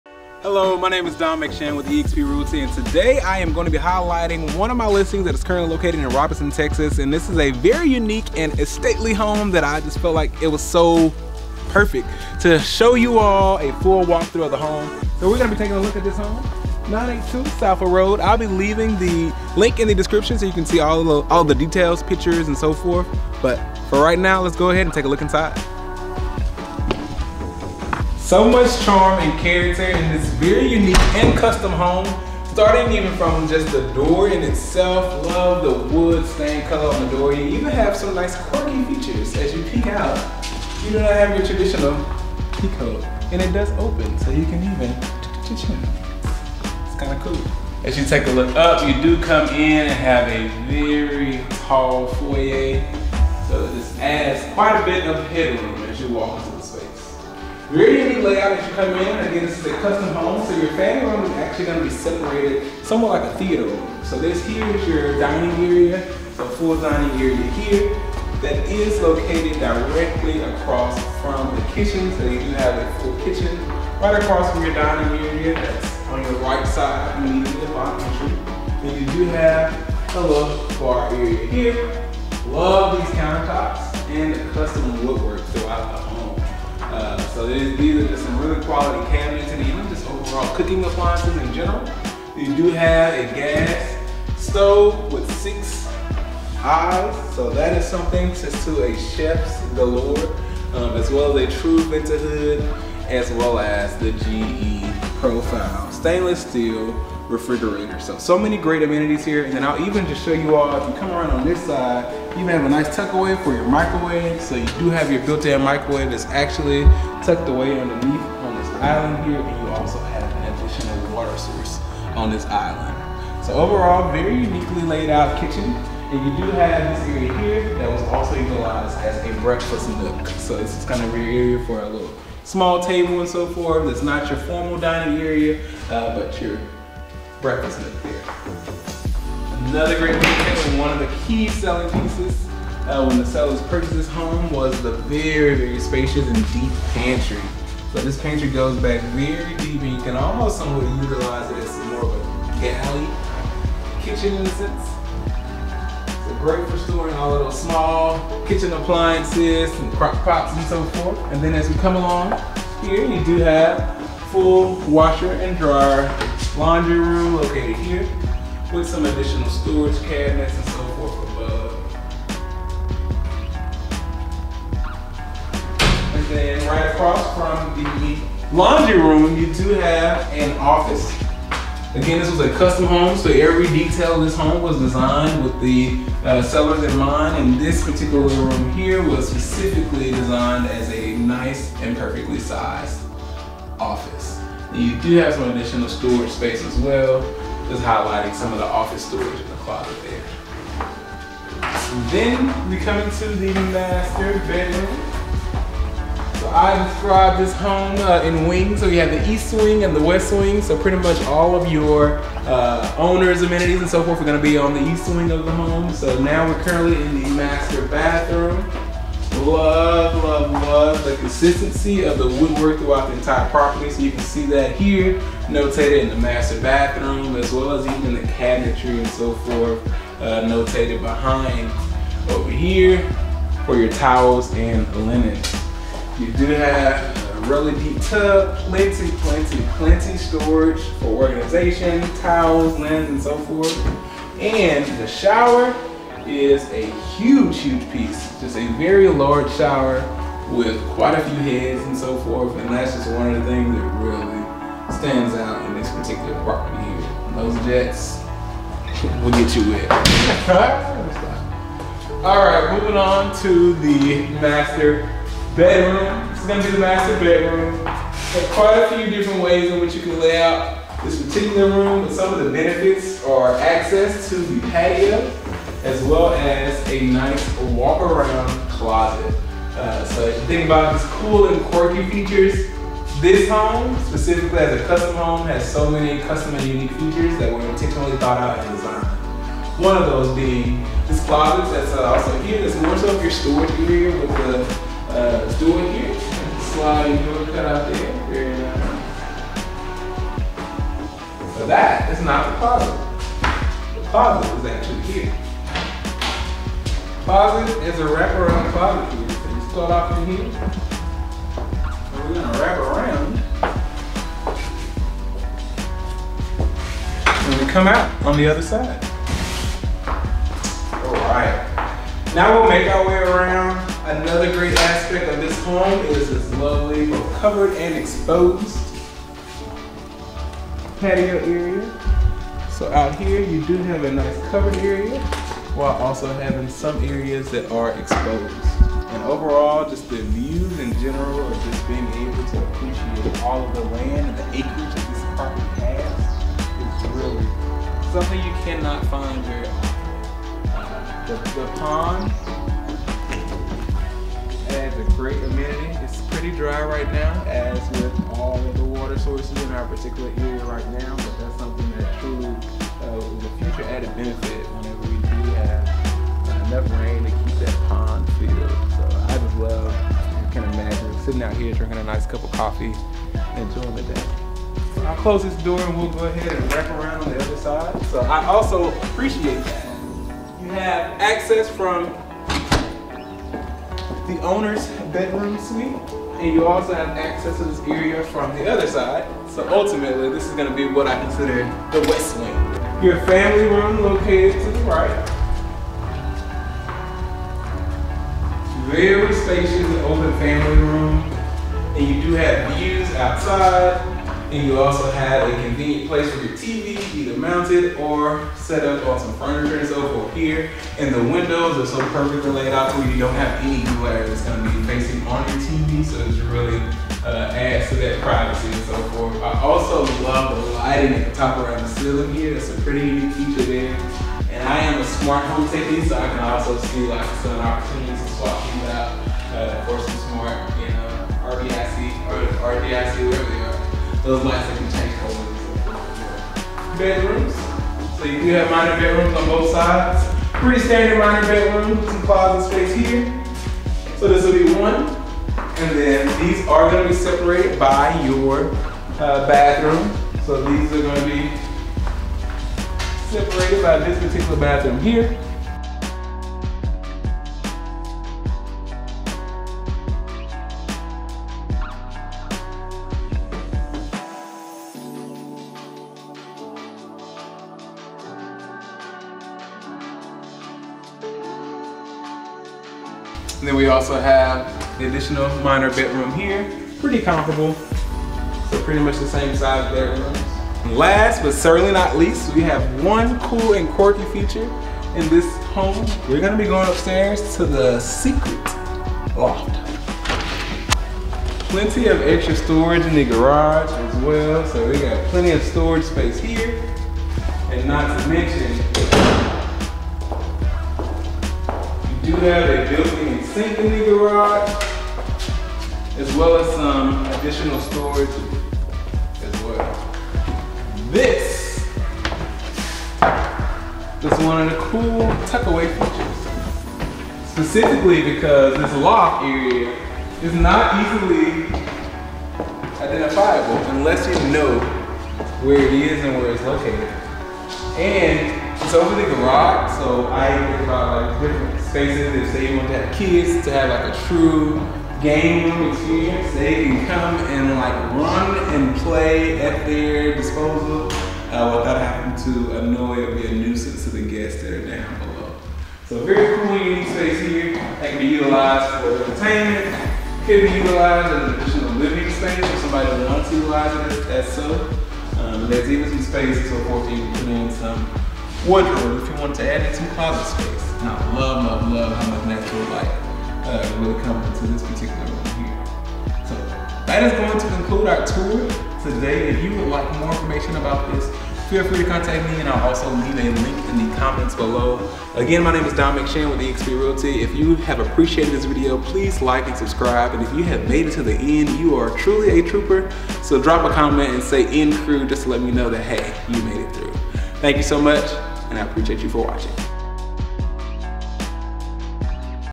For Sale | 982 Saffle Rd, Robinson, TX | 3BR 2BA | 2,727 Sq. Ft | Game Room | Office | 2+ AC | Pond
Welcome to peace & privacy in this stately Hacienda with tons of character and charming Spanish architectural features.

🌲2.28 Acres | Private Pond | 🛌 3BR | 🚿 2.1BA | 📐 2,727 sq. ft.
🗄 Office |  🎱 Game/Bonus Room

For pictures, pricing and more

Nestled on 2.275 acres in Robinson ISD, this Energy Star Qualified home is energy efficient with a 250 gallon propane tank to fuel gas-top cooking, the tankless water heater, and other essentials in the case of inclement winter weather (I.e. snowmageddon Haven).

Featuring 3 spacious bedrooms and 2 full baths, the home also boasts a private office PLUS an additional isolated bonus room on the second floor. The bonus room could easily be transformed into a home theater, guest quarters, a fourth bedroom, or a spacious studio space. The expansive two-car garage includes a sink, additional storage and work space as well. And if that weren't enough storage space, there is an additional 40-foot cargo trailer tucked in the back corner of the property--perfect for storing lawn and garden equipment.

Enjoy quiet evenings sitting on the dock of your own private pond, cast a line from the bank, or simply gather on the back patio and watch the Texas sunset. Additional details that should not be overlooked include space-saving pocket doors, extra deep window sills, GE Monogram appliances, recessed and ambient tray lighting in the living room, and a wall of spray heads in the master shower. This home was constructed in 2010 with insulated concrete forms, adding an unparalleled level of energy efficiency, and the foundation was reinforced with exterior and interior steel piers in November '21 with a transferable warranty. Call today to schedule your private tour and make this beautiful estate your home.

🔹 Exclusively Listed by
Dom McShan, REALTOR® l eXp Realty, LLC
🔹Your Astute Agent™️